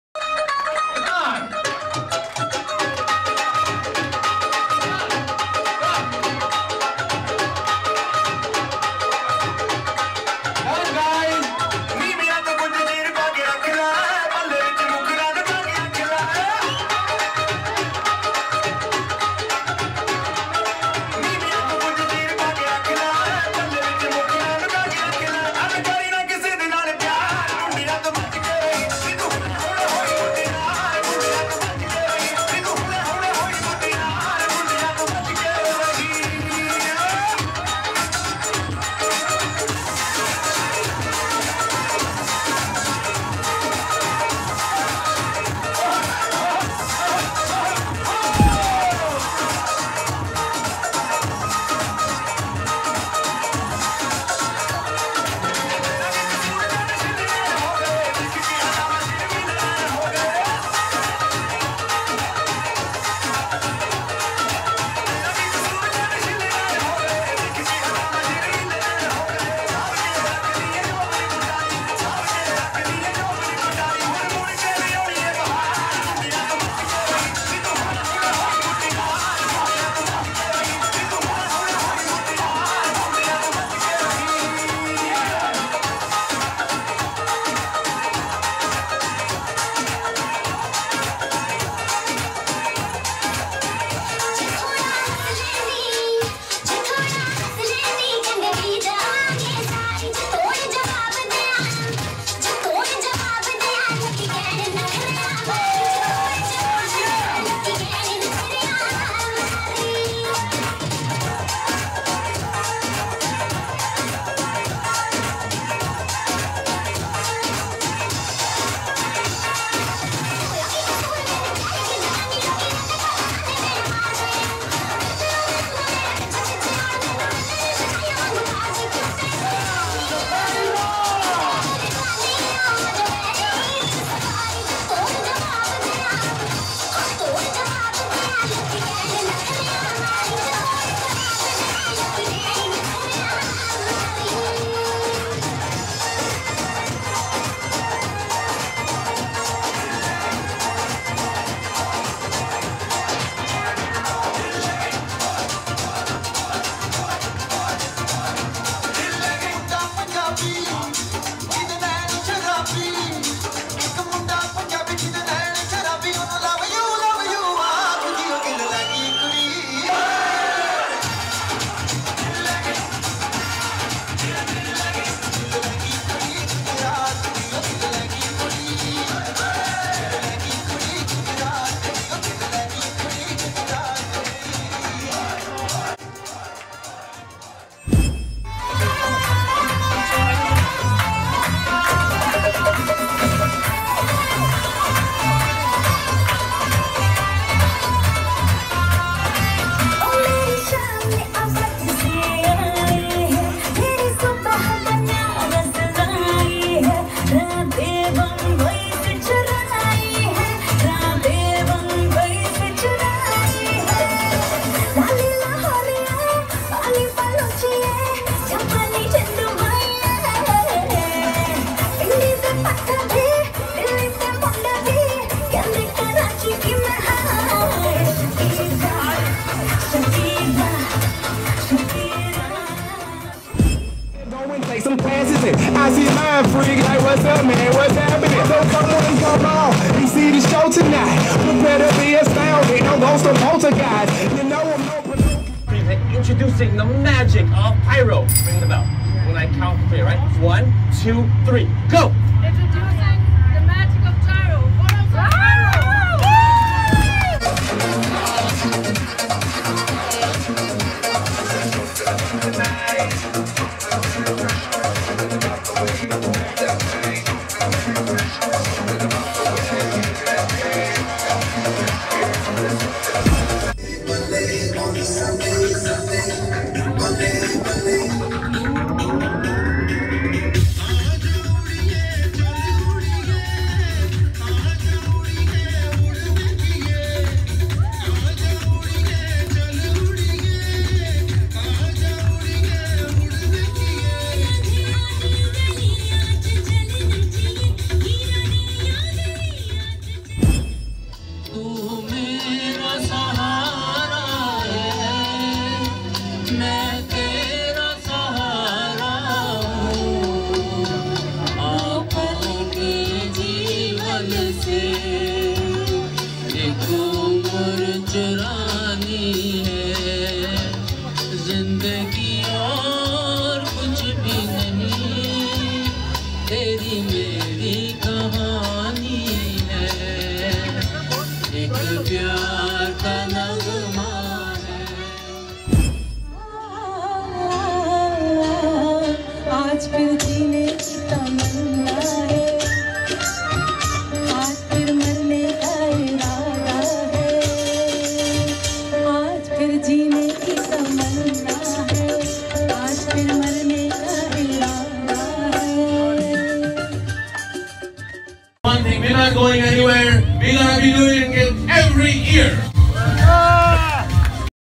Eid Cruise New York 2016
Eid cruise New York 2016 organized by Saeed Hassan and Bashir Qamar.